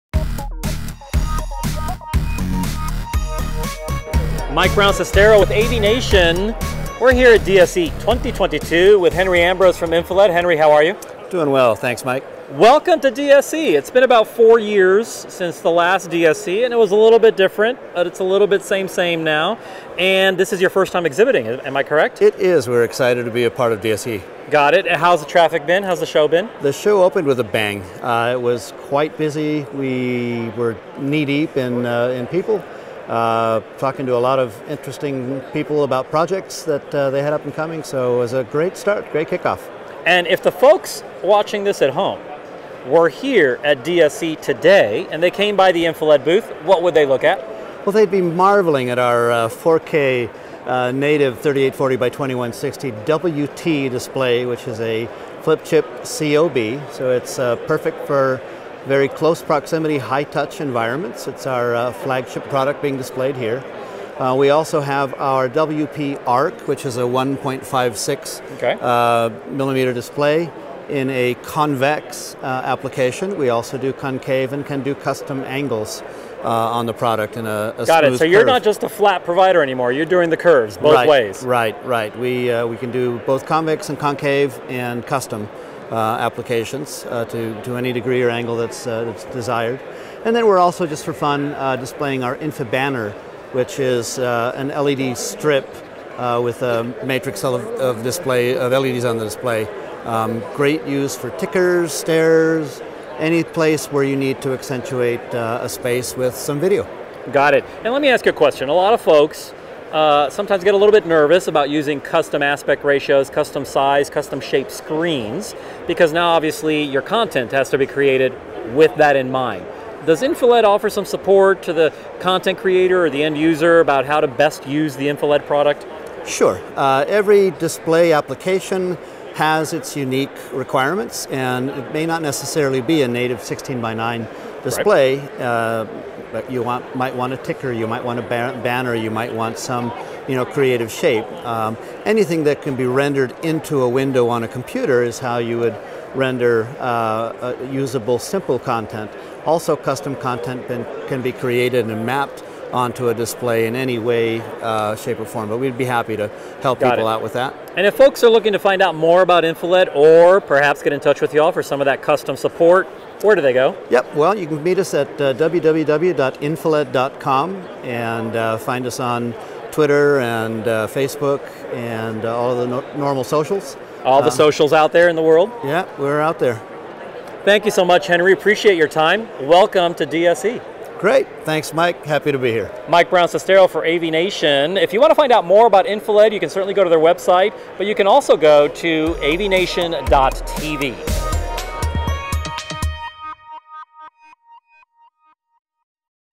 Screens Around Corners With INFiLED | DSE 2022
We are back in person for DSE 2022, looking at the latest innovations for digital signage. We talk to INFiLED's Vice President of Business Development Henry Ambrose to discuss their new products, from the massive 4K WT series to the convex screen capable of wrapping around corners.

Get all of our shows, stories and more on AVNation.TV

Check us out on different parts of the web!